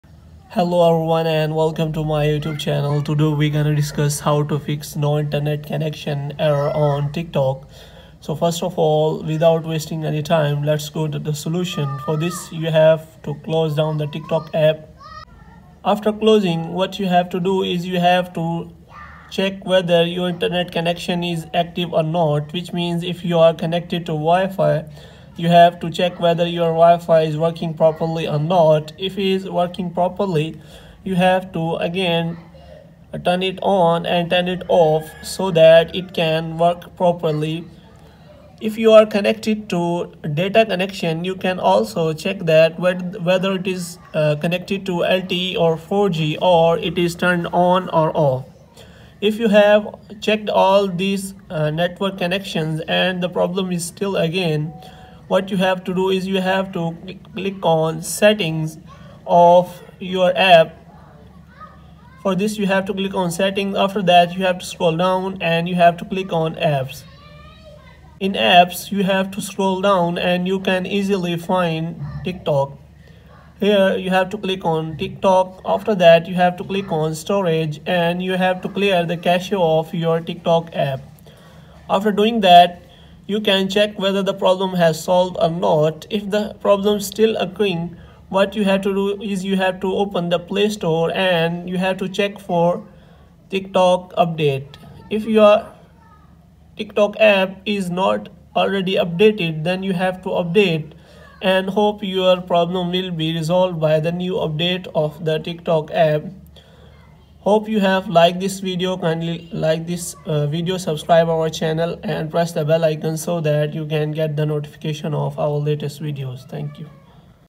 How to Fix TikTok No Internet Connection|How to Fix TikTok No Internet Connection 2023
Sometimes you were scrolling on TikTok and you face the error of no internet connection. If you are looking for how to fix tiktok no internet connection then you are on the right video. In this video we will tell you how to fix tiktok no internet connection on android. So if you want to know how to fix tiktok no internet connection on android you have to watch this video completely.